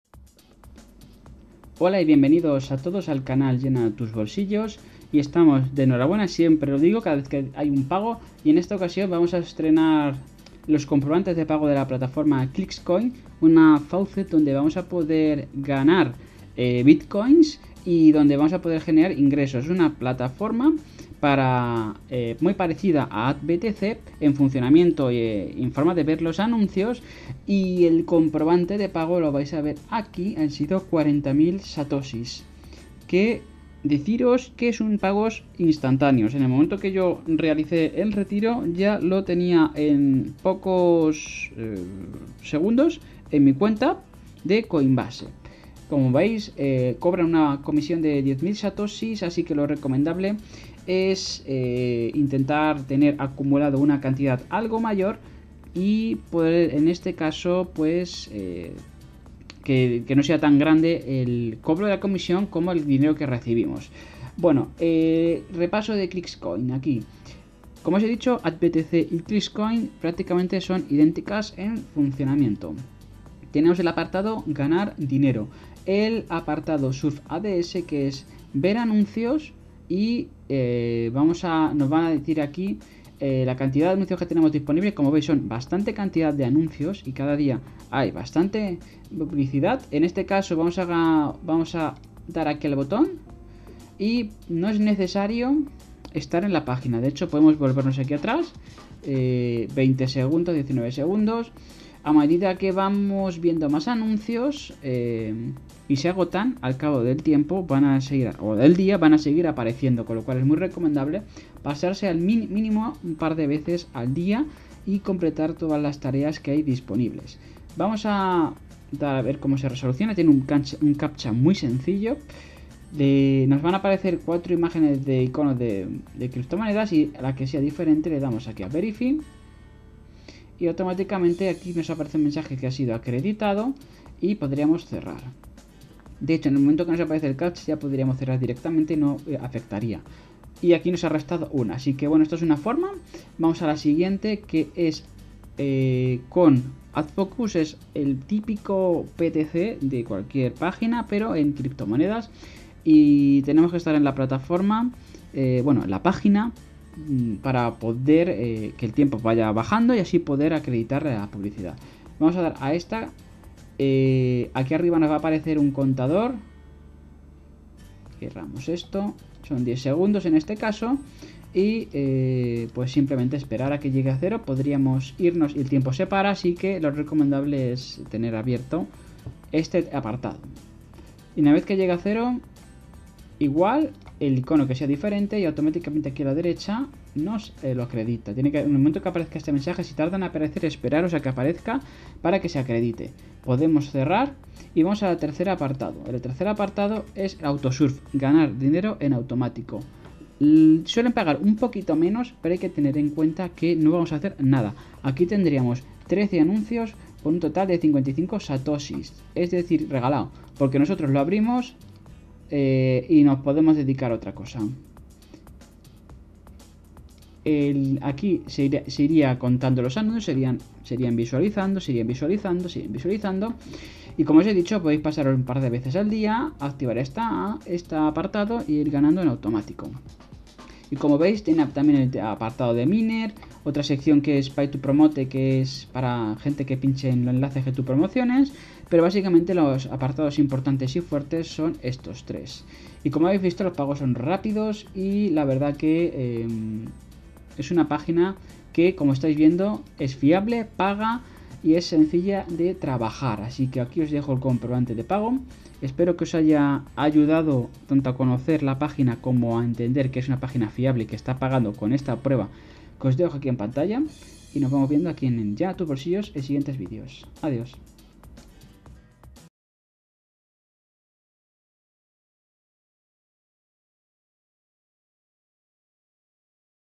Segumos sumando ganancias con las faucets | Nuevo comprobante de pago
🔴  NUEVO VÍDEO   🔴

Nueva prueba de pago de una plataforma faucet para ganar Bitcoins gratis desde casa, se llama Clixco.in y es uno de los negocios donde más satoshis se pueden ganar.

Las criptomonedas han venido para quedarse y no debemos desaprovechar las oportunidades de futuro que nos van a dar.